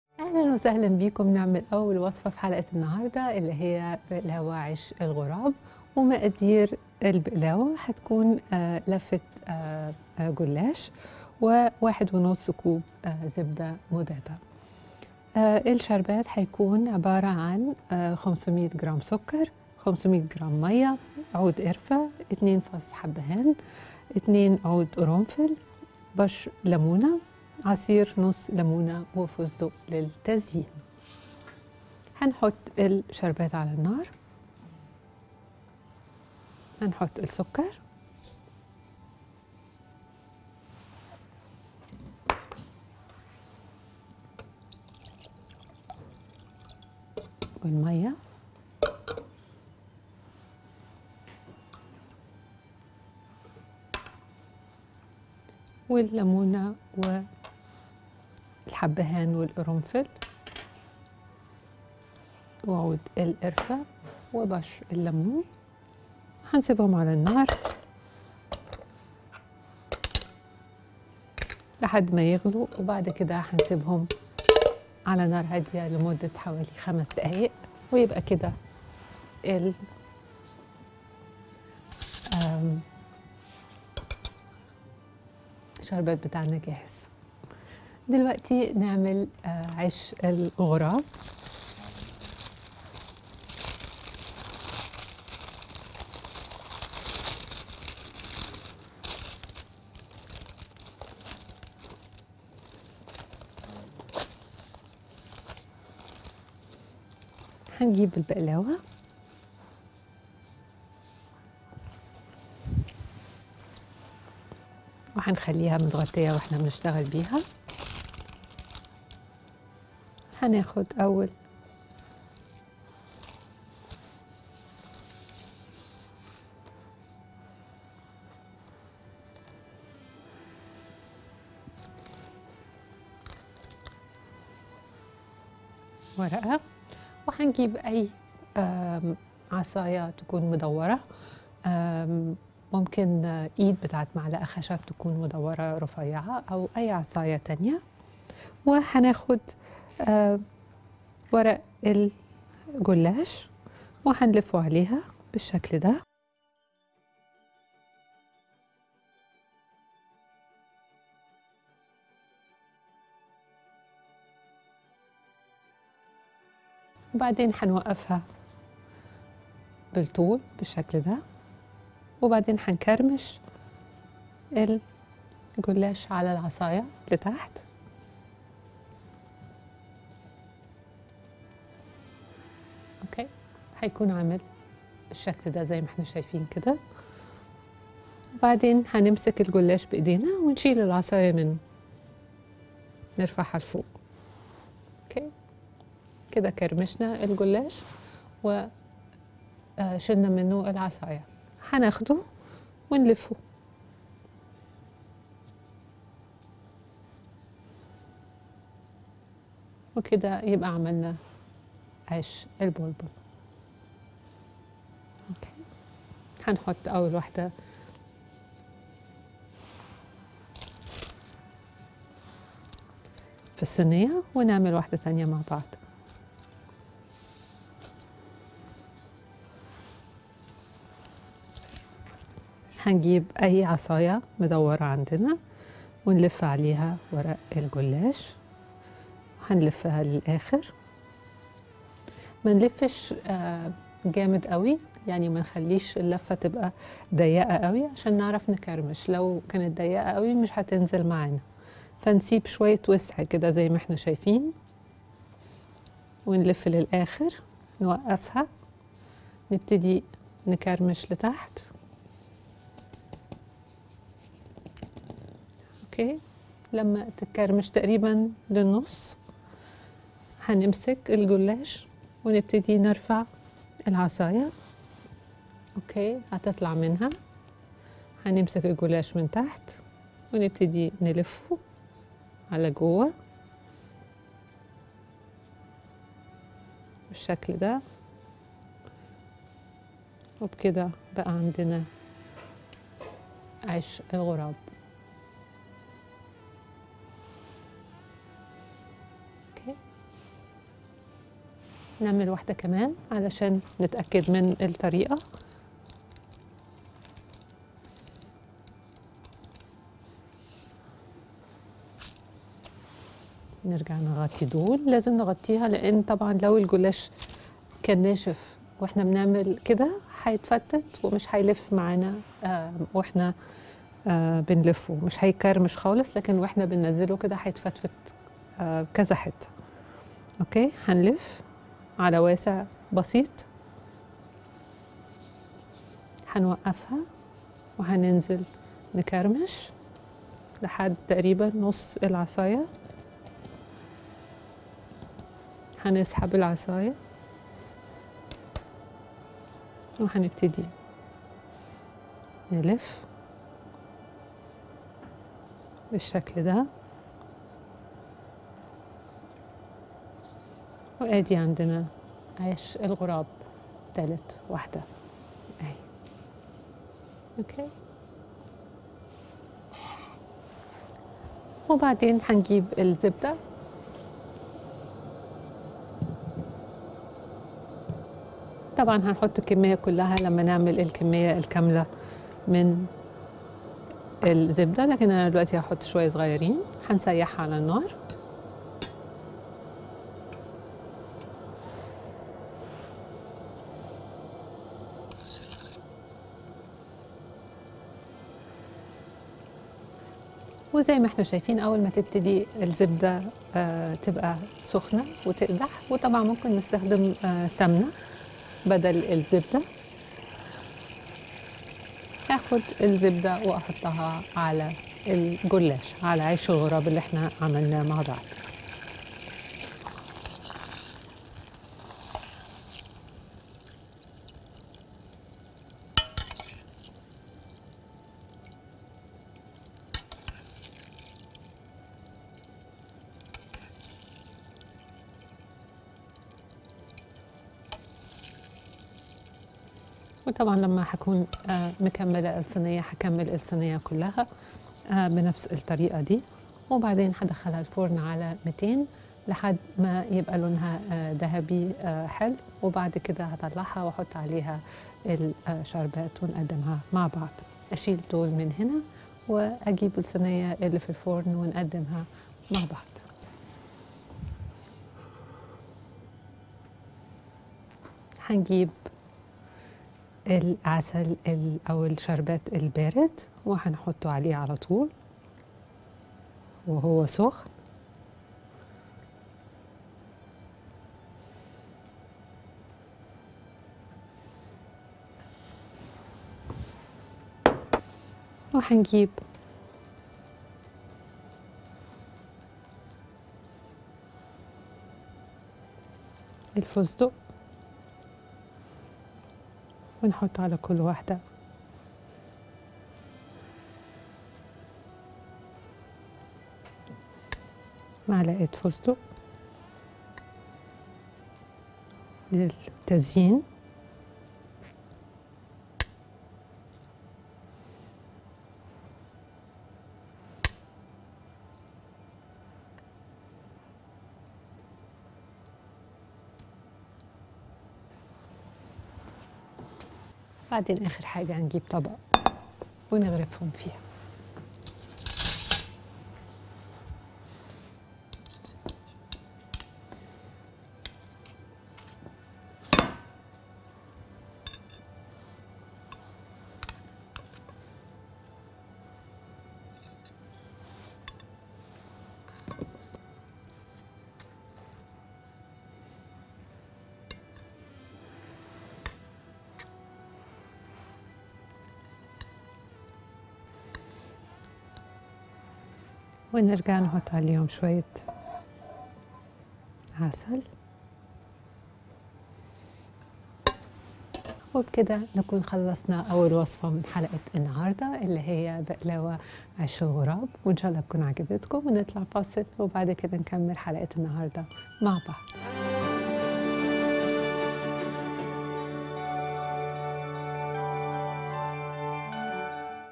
عش الغراب | نرمين هنو
طريقة عمل عش الغراب مع نرمين هنو في زي السكر